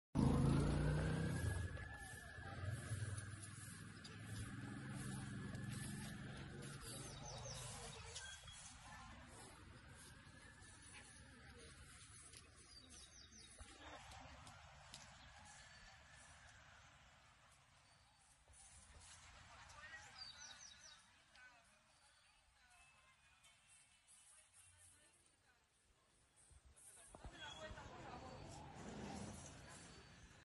Delincuentes robaron en una ferretería en la colonia Nueva San Salvado en Puebla
Delincuentes robaron en una ferretería y atacaron a balazos a policías, en la colonia Nueva San Salvador del municipio de Puebla. Los responsables huyeron en una camioneta y son buscados por los agentes.